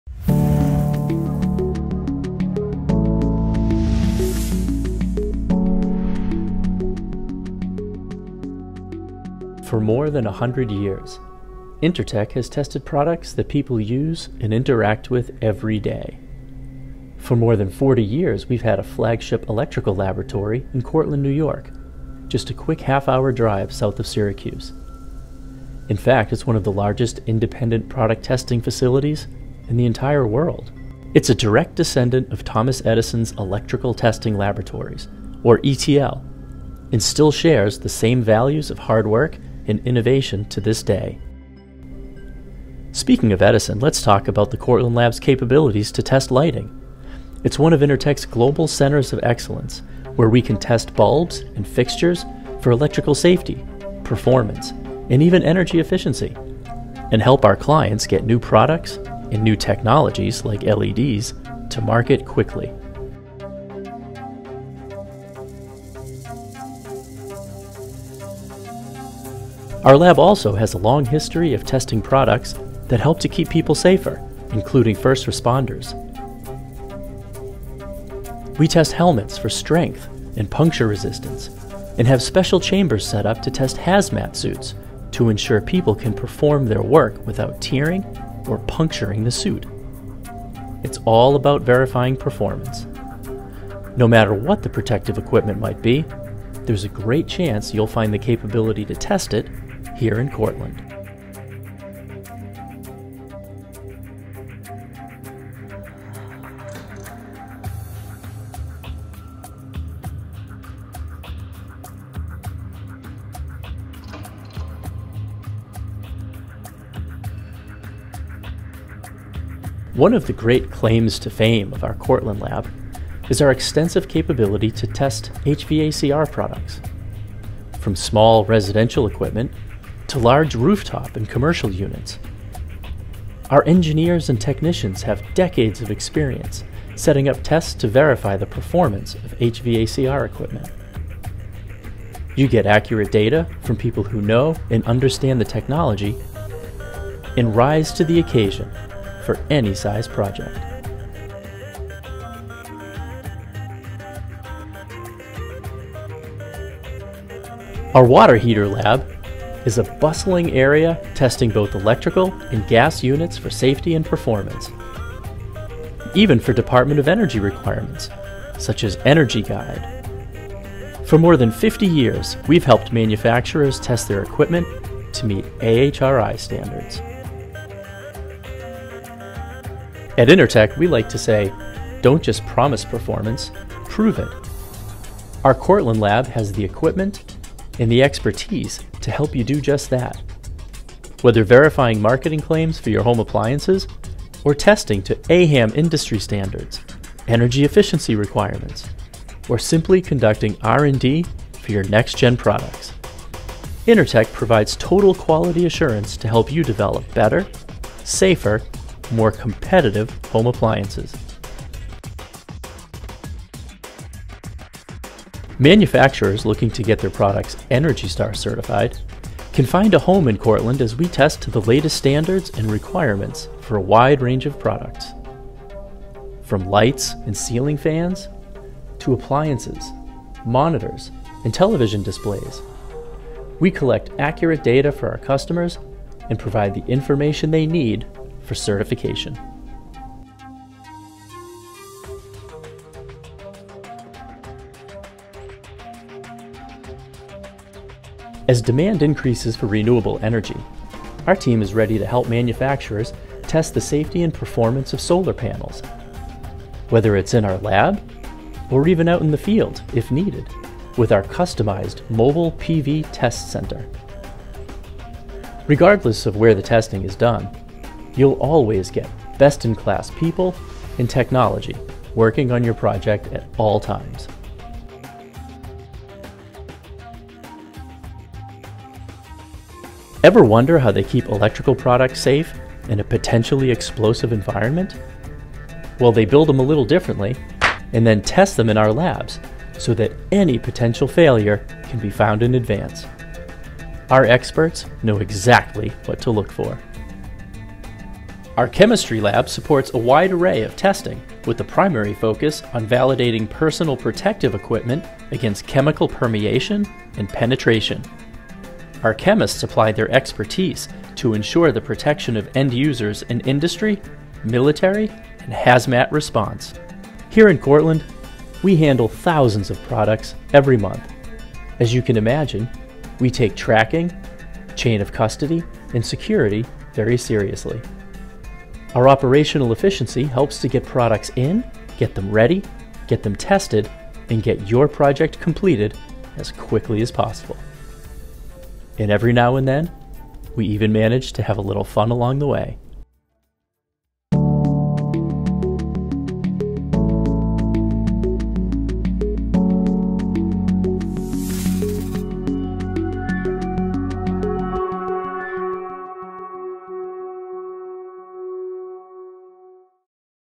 Video Tour of the Intertek Cortland, NY product testing laboratory
Learn more about Intertek's testing capabilities in our Cortland, NY laboratory. This lab specializes in Electrical products, including HVAC, Appliances, Solar Energy, Industrial equipment, Lighting products, and more, but they are also a worldwide leader in testing PPE equipment and chemicals. They conduct electrical safety testing per OSHA and National Electrical Code requirements and help manufacturers gain the ETL certification mark as proof of compliance with UL standards in the USA or CSA standards in Canada. They also conduct product performance testing per industry or client specifications, energy efficiency testing for programs such as ENERGY STAR® and the California Energy Commission, and much more. NOTE: some tours may need to be scheduled after COVID-19 travel restrictions to the state of New York are lifted.